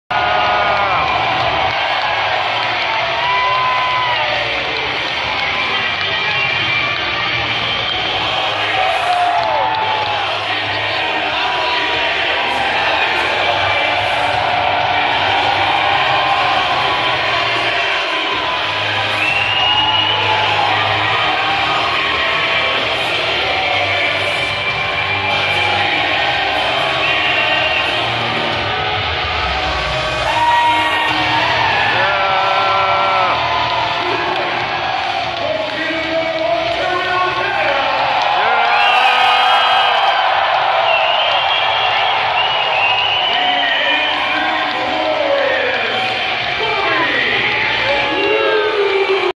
Bobby Roode returns to Peterborough 2018
Robert Roode makes a special appearance to his hometown in Peterborough at the memorial Center. Simply Glorious!!